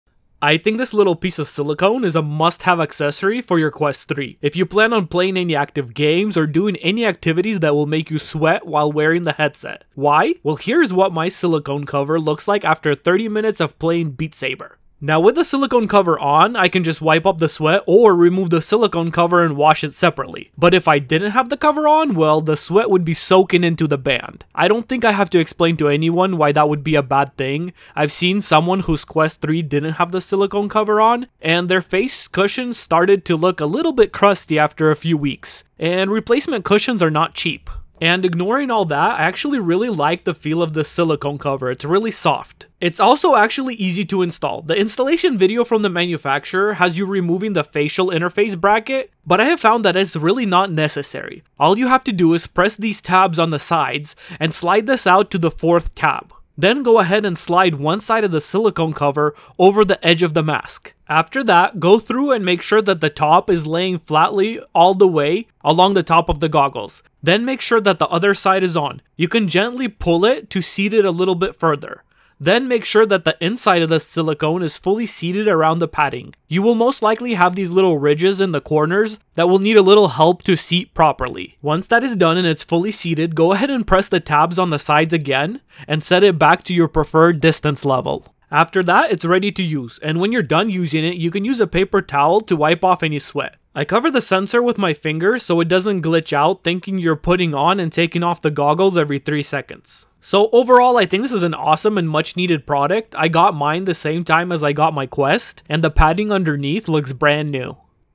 Must Have Quest 3 Accessory
This in my opinion is a must have accessory for your quest 3. To help protect your VR goggles cushion from sweat. I think the APEXINNO VR Silicone Face Pad Cover for Quest 3 does a great job protecting your device.